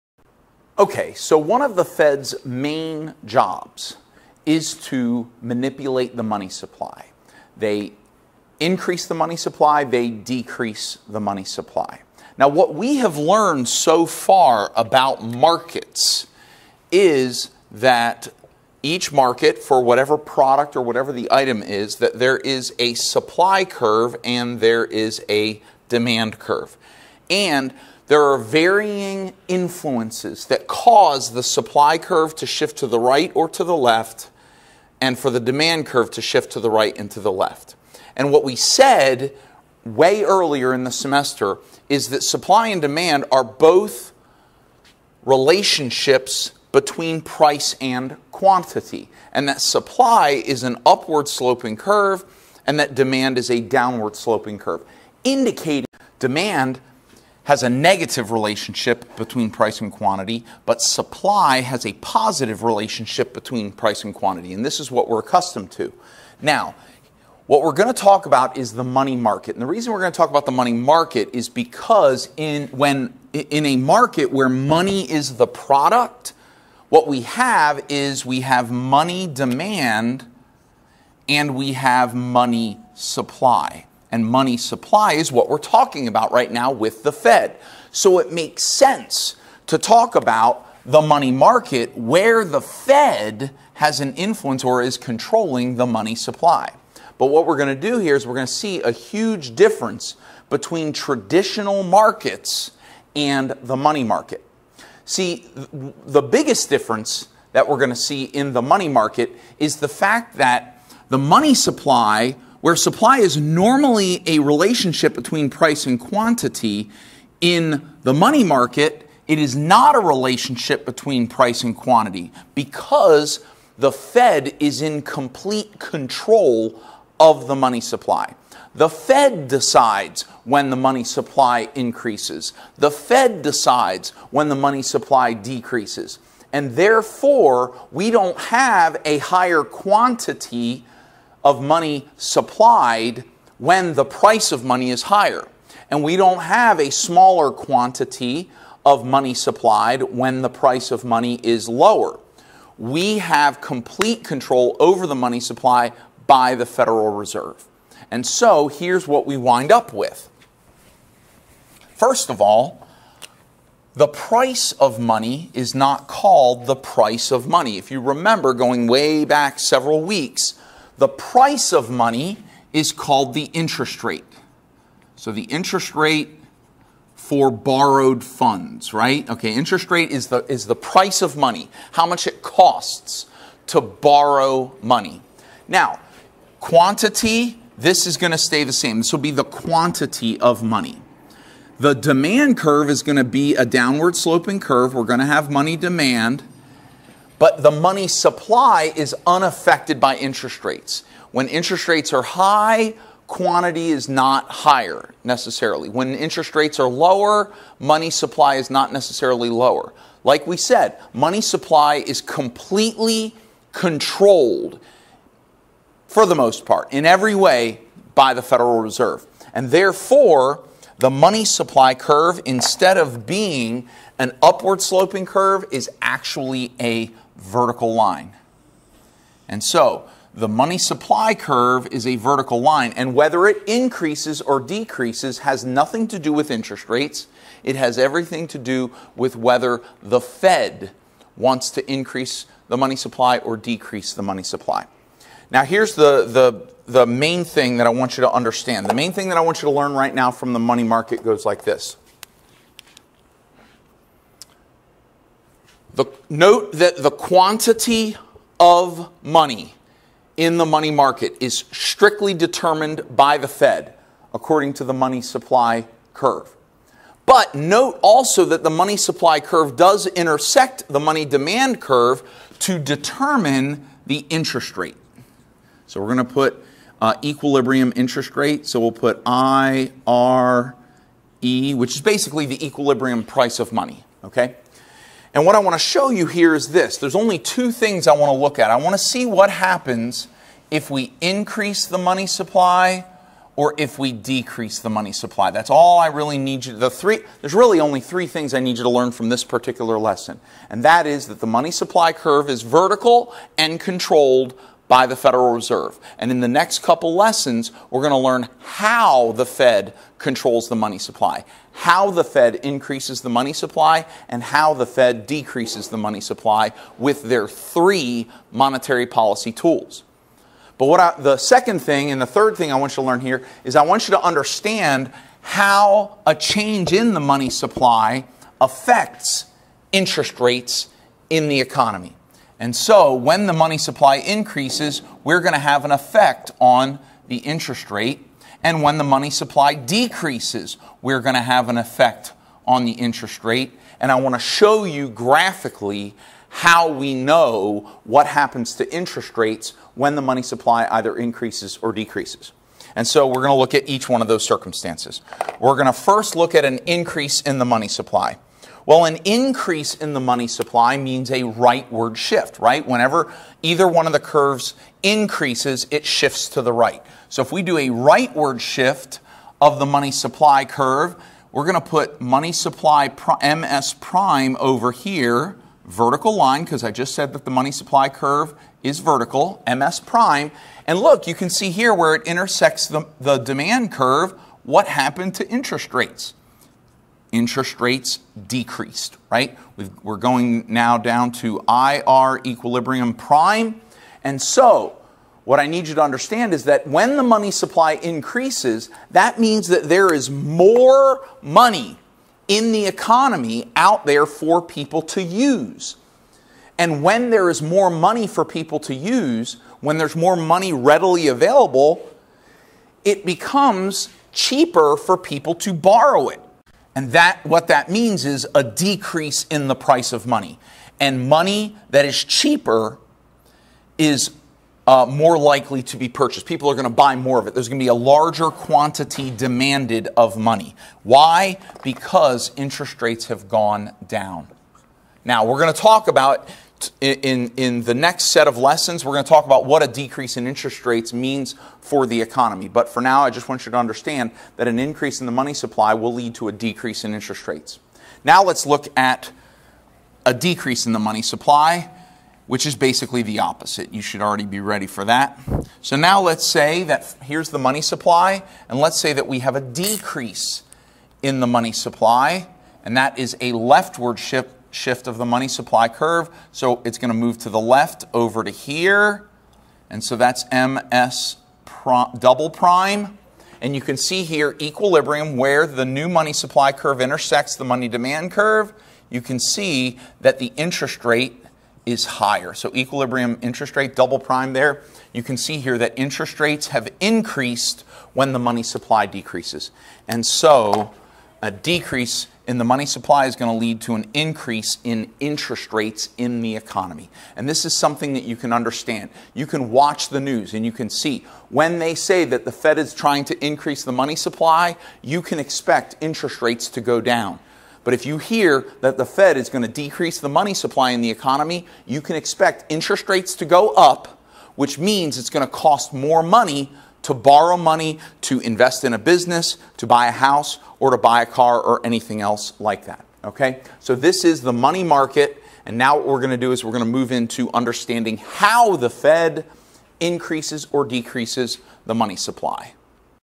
The Money Market - Professor Ryan
Professor Ryan explains the money market with emphasis on the supply of money as determined by The Fed.